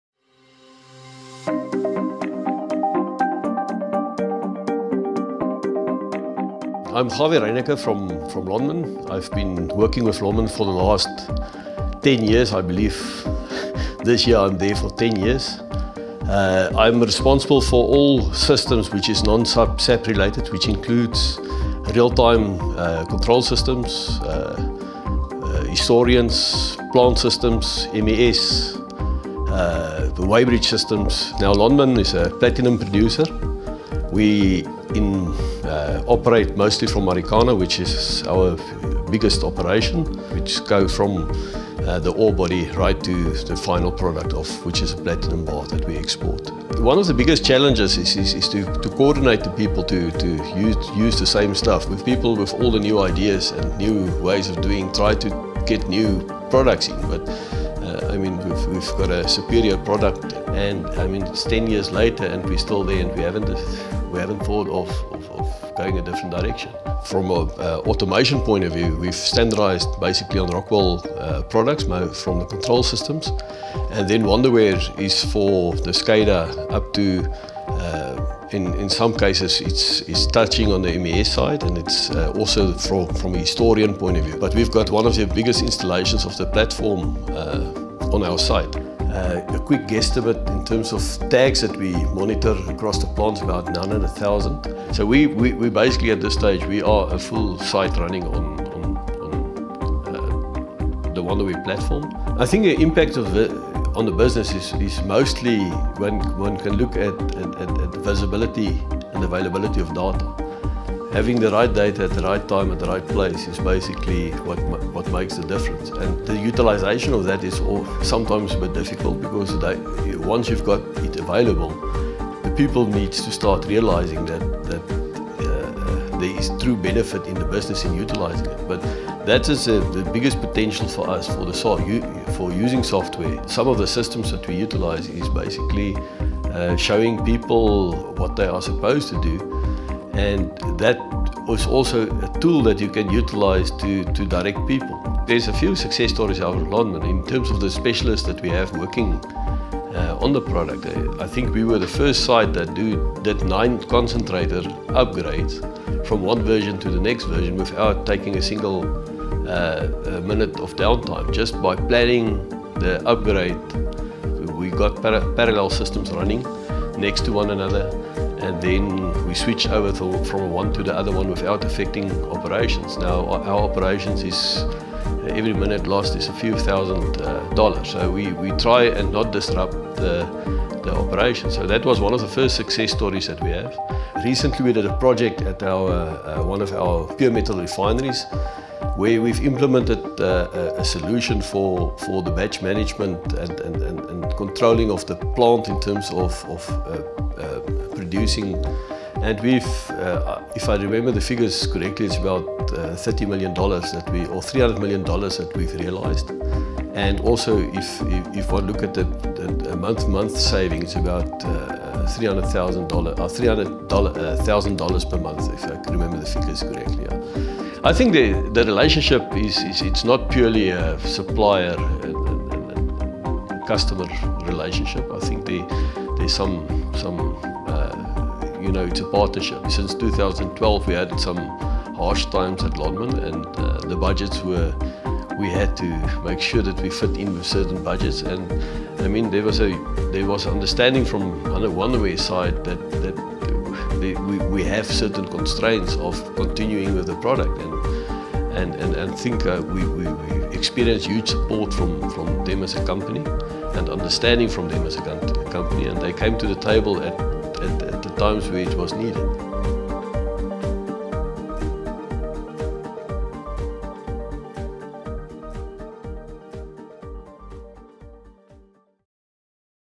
Wonderware in Action: Lonmin - Mining Success
Helping one large-scale mining operation transform and visualize raw data into actionable knowledge.